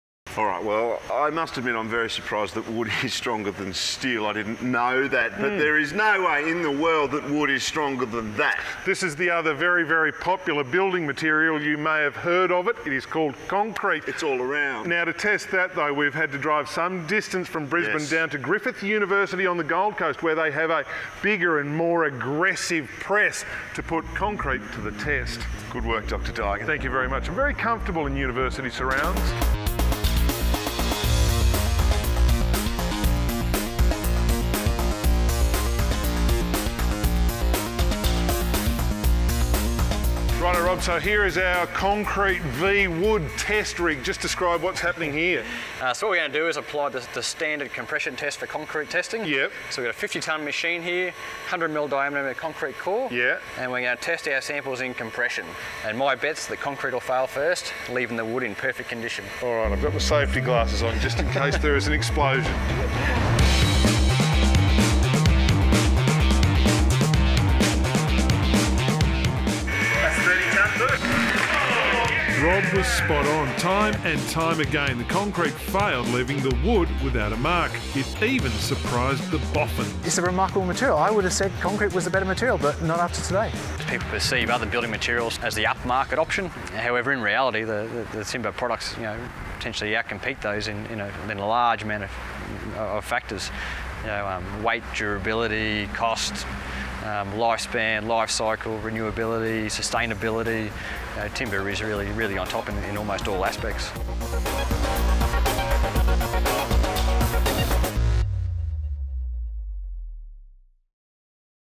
Wood vs Concrete - the Material Difference
Is wood stronger than concrete? Join Nick and Andrew as they apply tonnes to discover an answer than may surprise you!